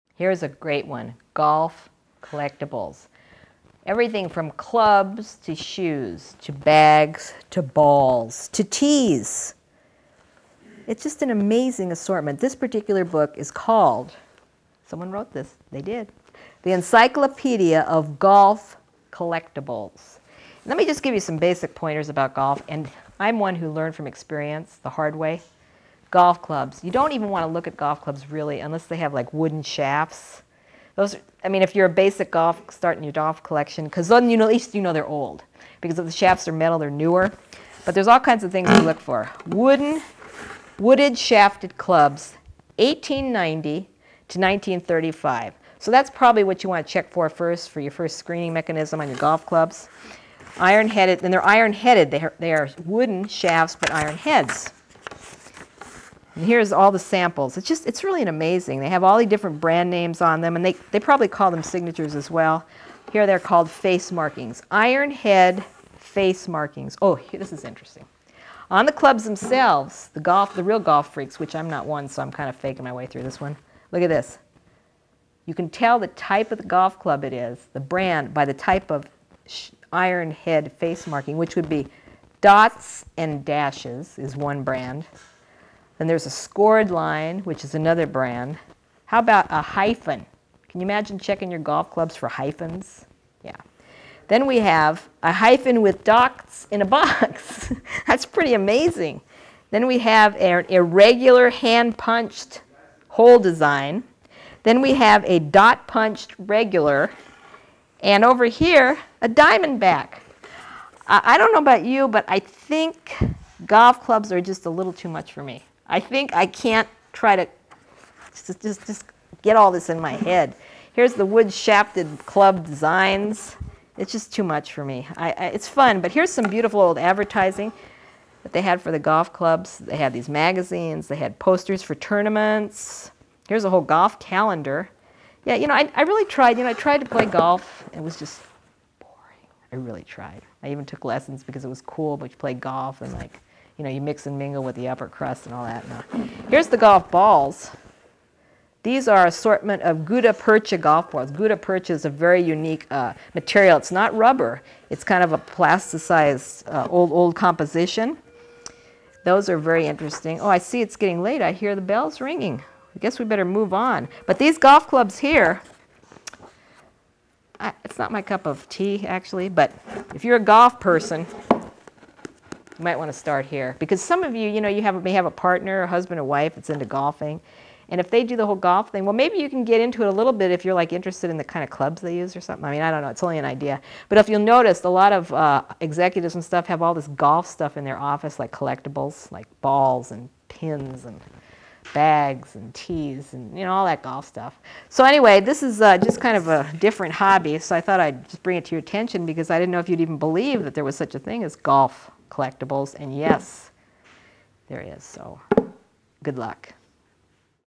Collecting Antiques : Antique Golf Collectibles
Using The Encyclopedia of Golf Collectibles as a reference, collect a variety of golf antiques, such as shoes, clubs, balls and tees. Learn more about collecting golf antiques with expert tips from an antique appraiser in this free video about collecting antique memorabilia.

Expert: Jan Braunstein
Bio: Jan Braunstein owns and operates the Antique Avenue antiques store in Pomona, Calif. Her mother also owned an antiques store. She is a certified antique appraiser.